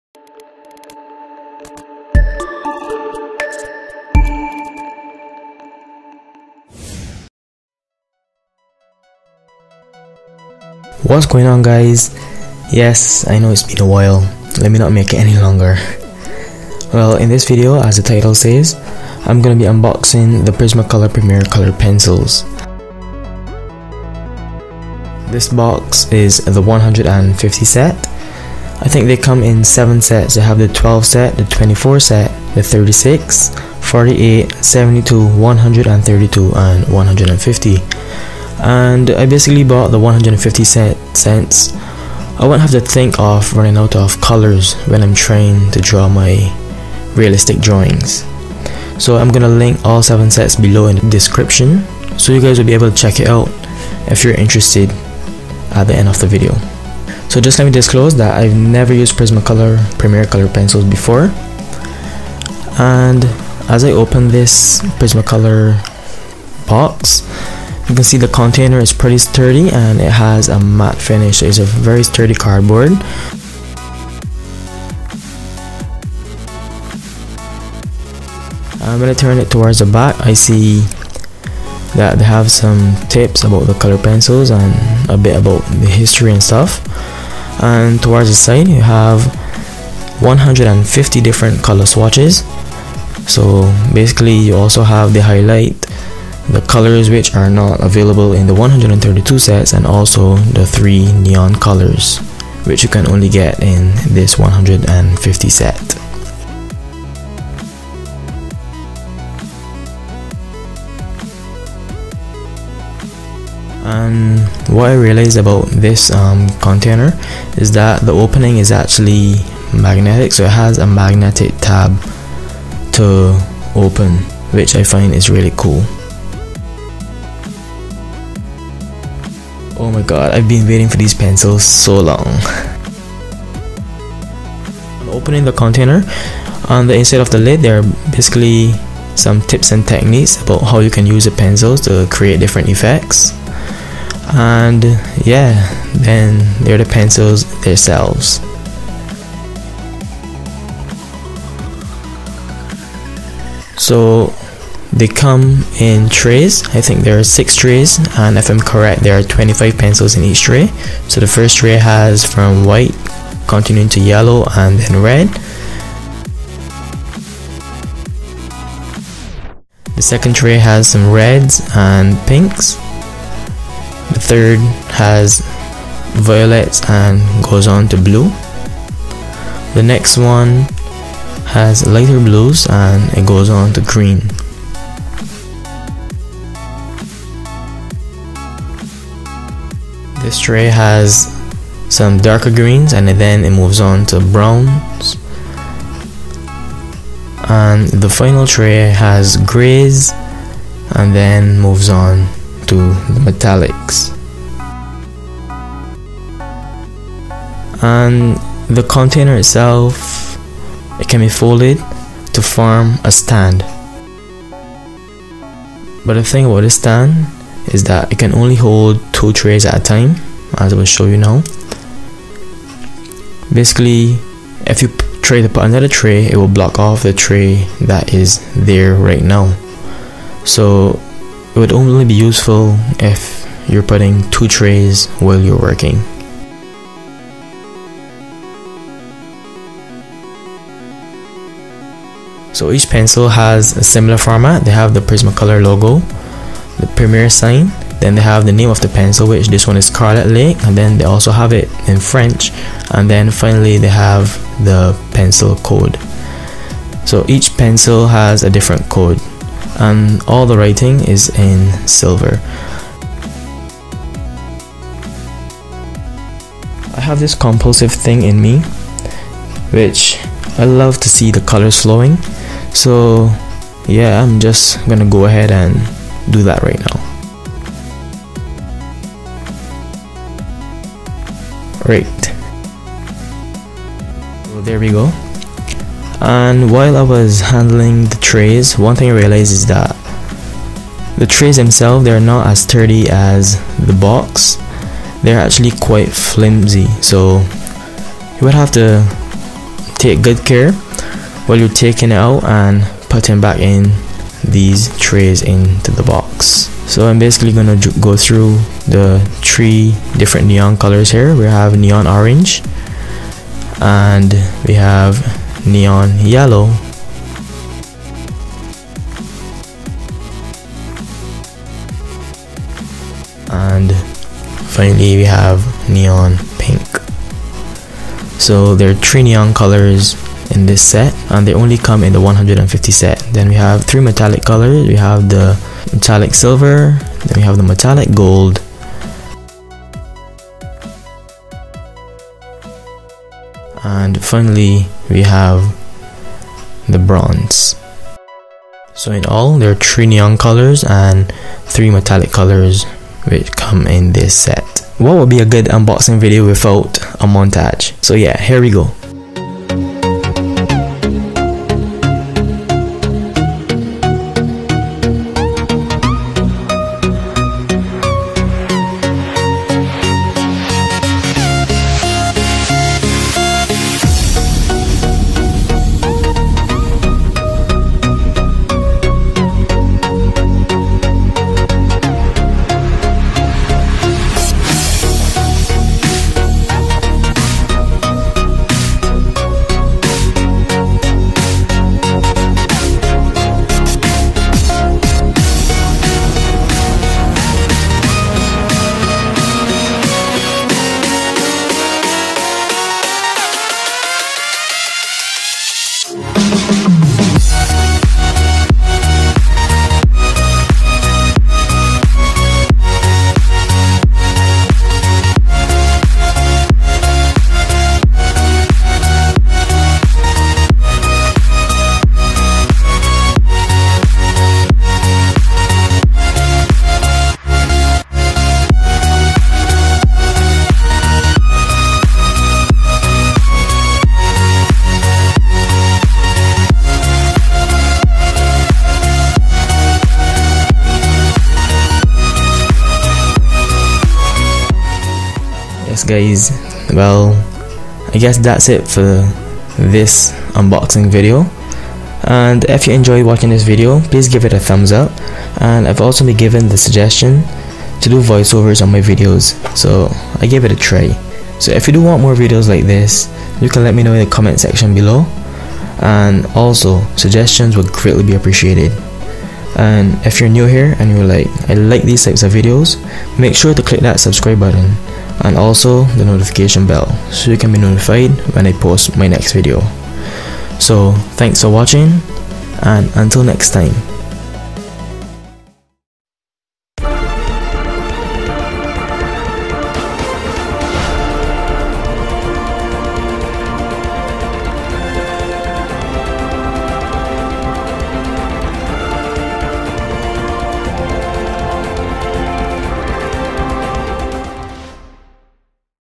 Unboxing Prismacolor Premier Colored Pencils (150 set)
Prismacolor Premier Verithin Colored Pencils

Strathmore 300 Series Bristol Smooth Pad

Strathmore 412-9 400 Series Toned Tan Sketch Pad
Strathmore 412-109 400 Series Toned Gray Sketch Pad

Hey guys, what's going on? This video was long due, but nevertheless here it is. This is my first unboxing video of hopefully many more to come. In this video I will be unboxing the prismacolor premier colored pencils 150 set. I've been awaiting these pencils for soo long now and I've finally got em. I hope you are looking forward to  seeing some upcoming drawings using these pencils.
This is also my first video where I tried adding my voiceover. Suggestions for drawing videos will also be greatly appreciated.